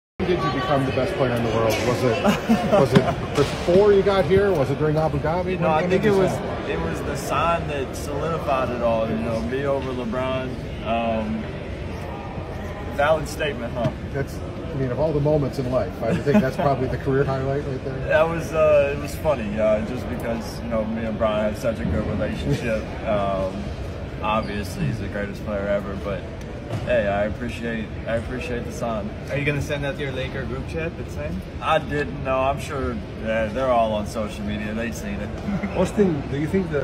Austin Reaves reacts to the “Austin Reaves over LeBron James” sign
Austin Reaves reacts to the “Austin Reaves over LeBron James” sign | via JK Carandang/GMA Sports PH

Follow GMASPorts for more updates on FIBAWC.